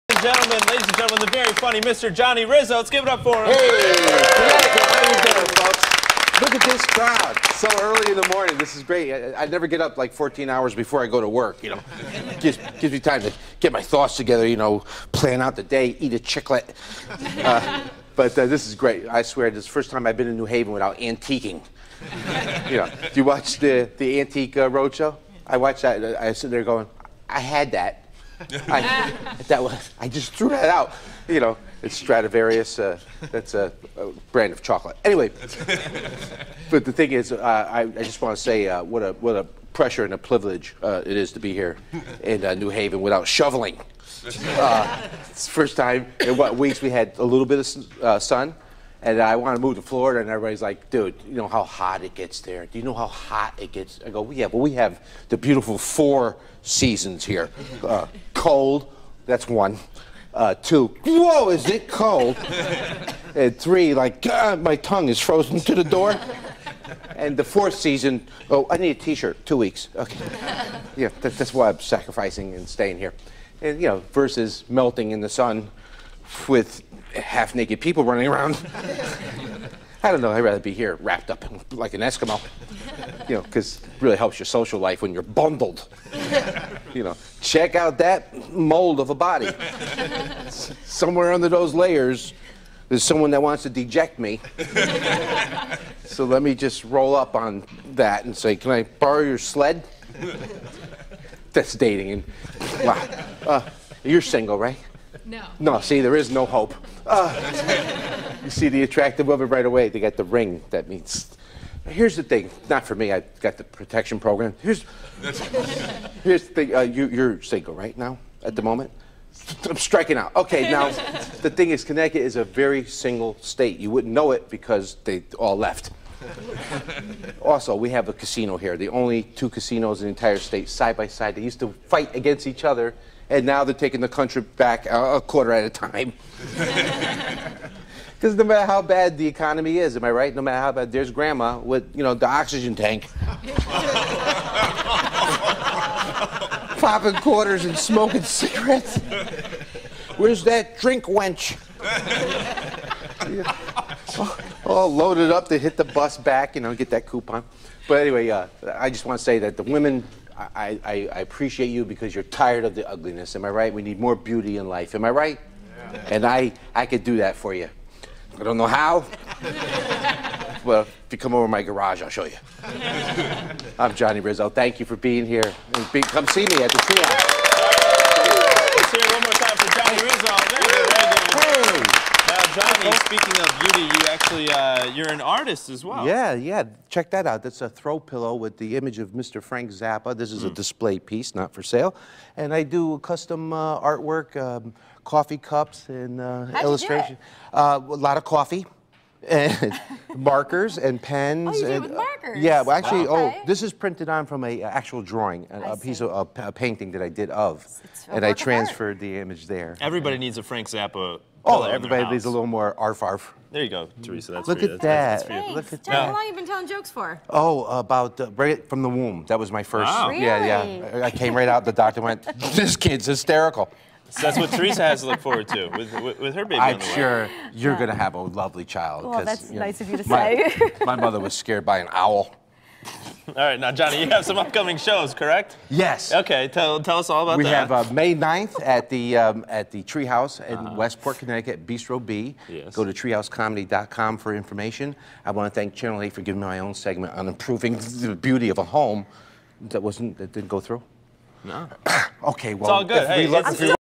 Johnny Rizzo Performs
Comedian Johnny Rizzo performs on Stage 8